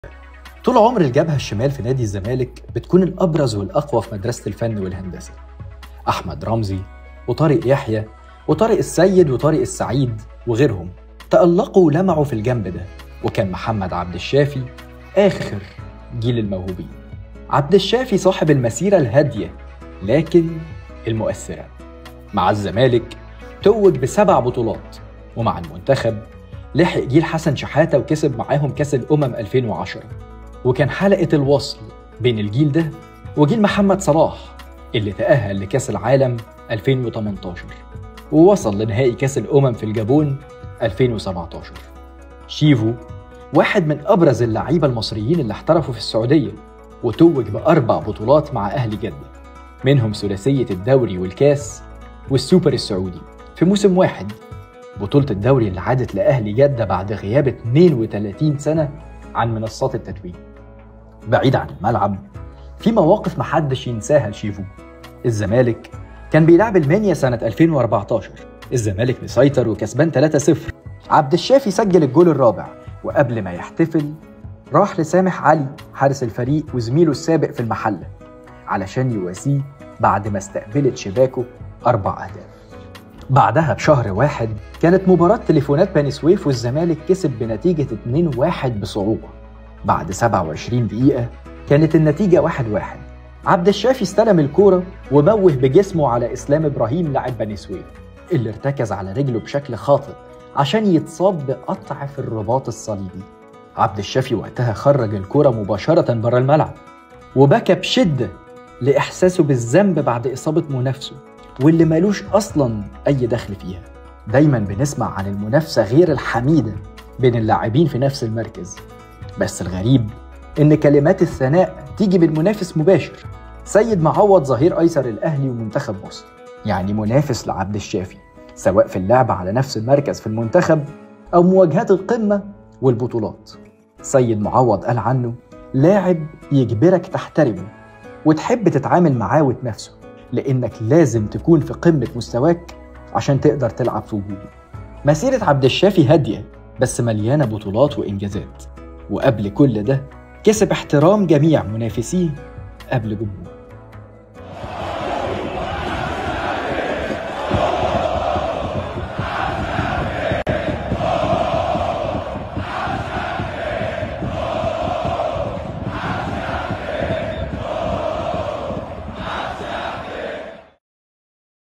بعد إصابته بقطع في الرباط الصليبي.. مواقف لا تنسى لـ عبد الشافي خلال مسيرته 👏
لا يوجد شخص أو منافس أختلف علي أخلاقه أو طريقة لعبه 🫡